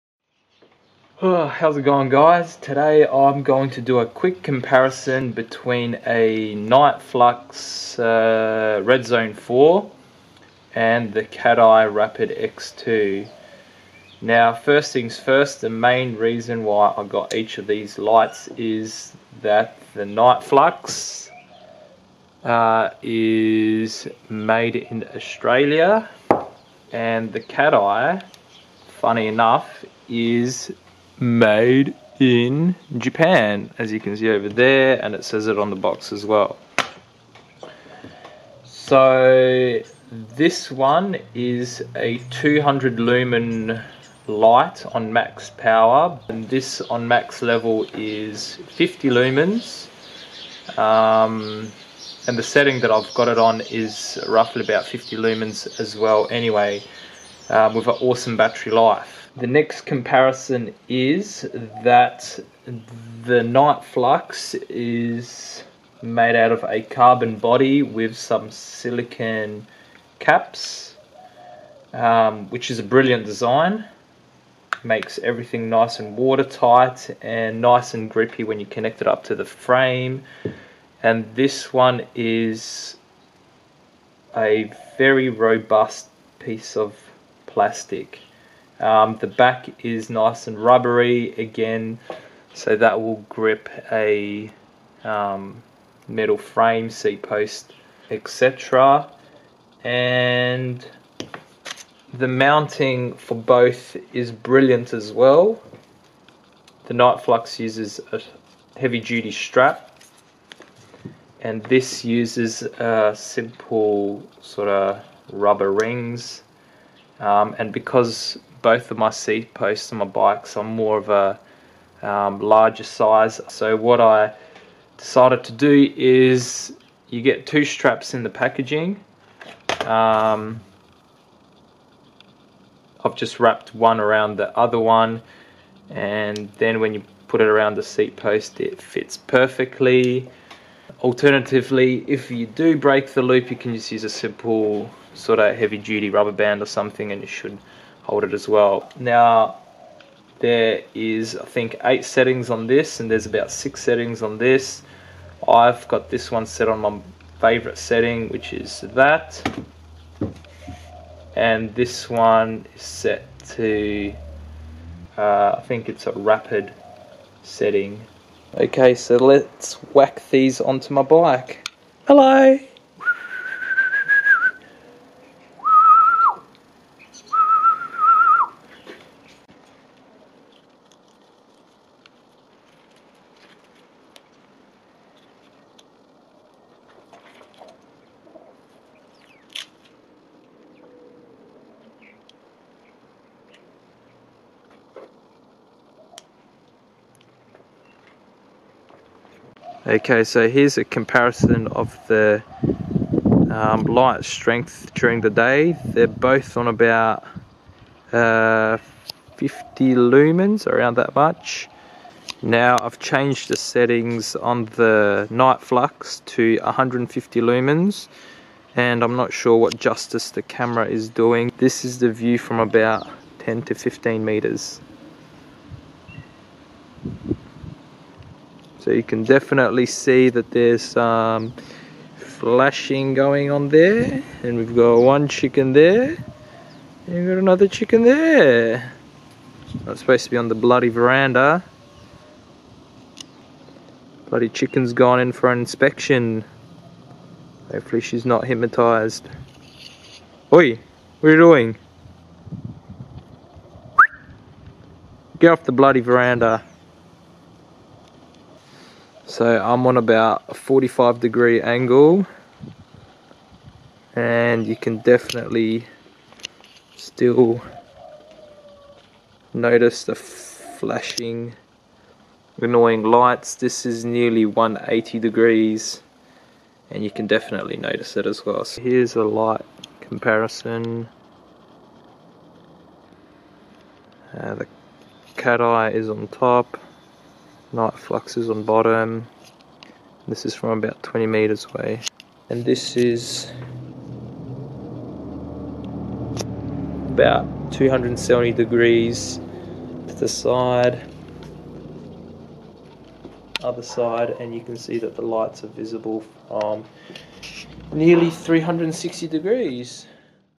Cateye X2 Kinetic VS Niteflux Redzone [ chicken approved ]
Rear light comparison between:
Cateye Rapid x2 and Niteflux Redzone 4

Cateye: 50 Lumens MADE IN JAPAN   $70

Niteflux: 200 Lumens MADE IN AUSTRALIA     $100

Both are different but brilliant in every way, the Niteflux is bigger, heavier and brighter but on the other side the size and quality of the Cateye is hard to match with any other rear light.

Another important feature of both the lights are bright enough for use during the day, rain or shine, both have highly efficient batteries/ electronic systems and both are noticeable from nearly 360 degrees.